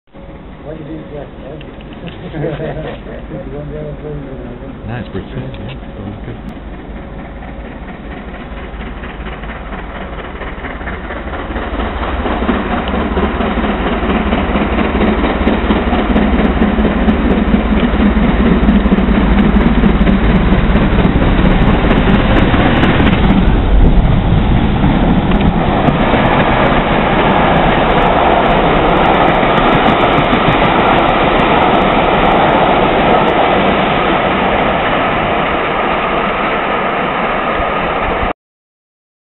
7000 Britannia
BR standard class 7 No: 7000 Britannia passing through Deepcut on the 19/7/11